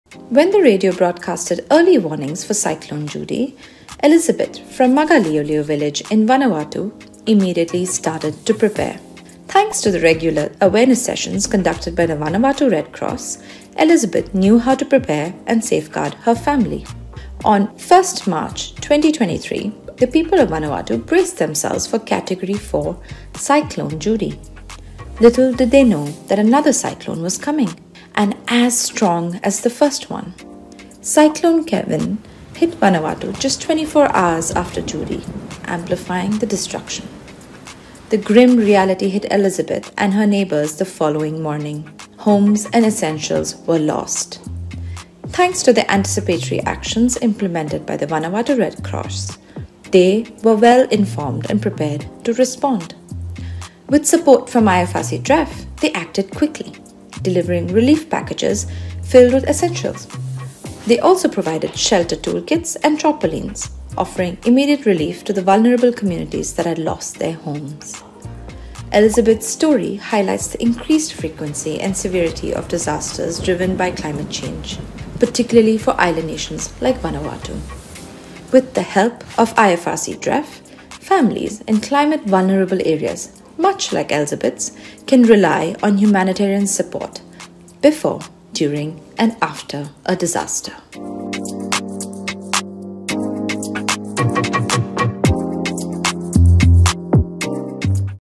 IFRC-DREF Disaster Spotlight- Vanuatu Double Cyclone
On 1 March 2023 the people of Vanuatu braced themselves for Category 4 Cyclone Judy. Little did they know that another cyclone was coming, and as strong as the first one. Cyclone Kevin hit Vanuatu just 24 hours after Judy, amplifying the destruction. Thanks to the anticipatory actions implemented by the Vanuatu Red Cross they were well-informed and prepared to respond.